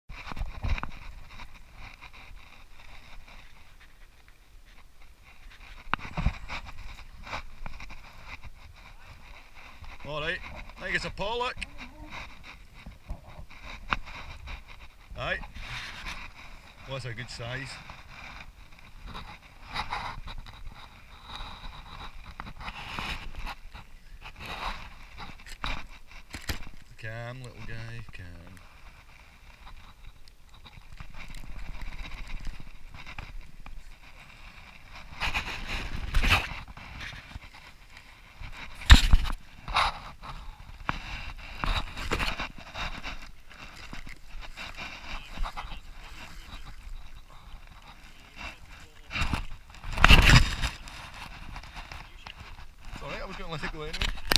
Catching a pollock from a kayak
Me catching a pollock from my wee kayak while out on the water between saltcoats and ardrossan.